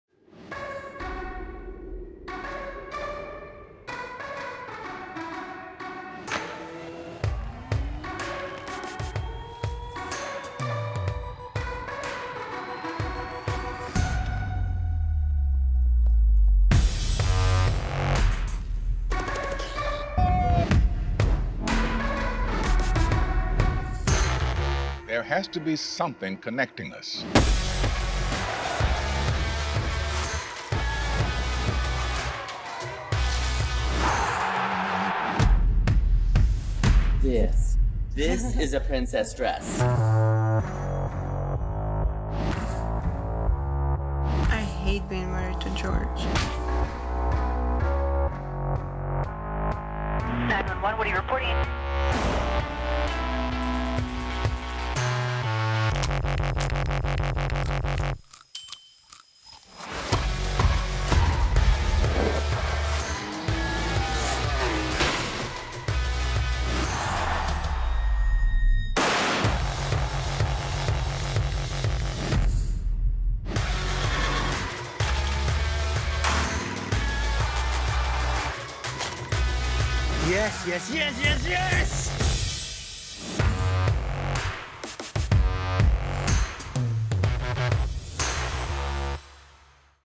This is: Discovery Networks Benelux!
Discovery Networks Benelux voert acht zenders in Nederland, waaronder  Discovery Channel, TLC, Eurosport en ID.

Discovery Channel, dé non-fictie mannenzender bij uitstek, is autoriteit op het gebied van alle onderwerpen die betrekking hebben op de wereld om ons heen.

TLC is dé non-fictie en lifestyle entertainment tv-zender voor vrouwen.  TLC is fris en anders en vertelt mooie, ontroerende, en altijd oprechte verhalen over bijzondere en kleurrijke mensen.

ID is dé crime zender vol spanning, drama en mysterie. De zender vertelt waargebeurde verhalen over waar mensen toe in staat zijn als ze tot het uiterste worden gedreven.

Eurosport combineert al sinds 1989  kennis en passie in een aanbod van meer dan 100 verschillende sporten. Wielrennen, voetbal, tennis, Olympische zomer- en wintersporten, snooker en auto- en motorsporten vormen de kern van haar LIVE portfolio.